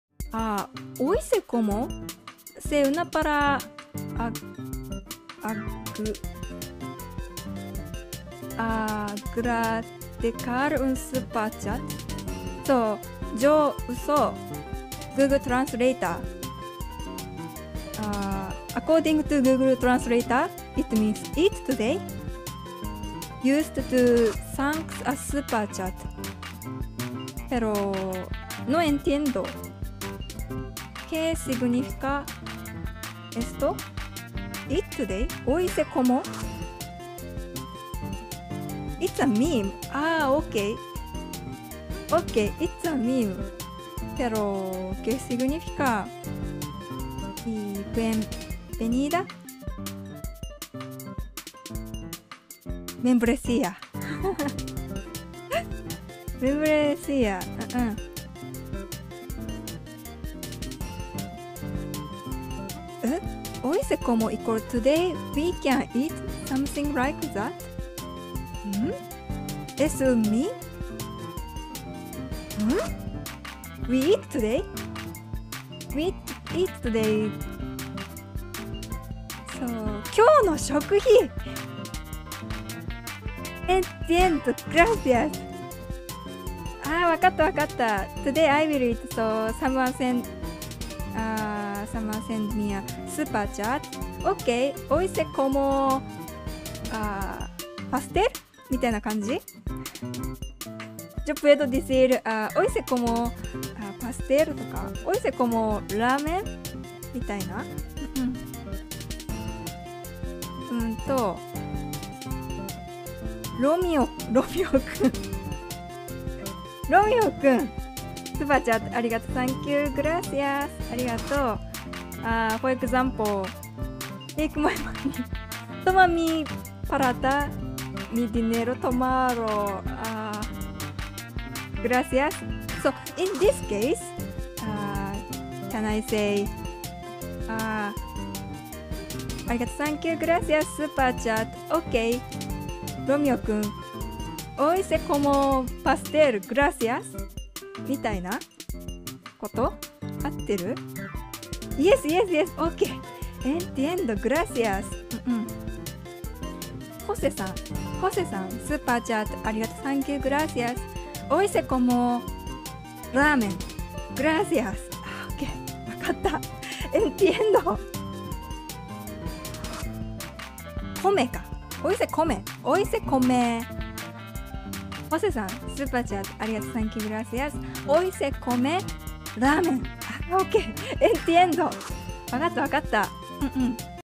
Kaichou aprende a usar la frase "Hoy se come" para agradecer los SC XD....
Link del directo del clip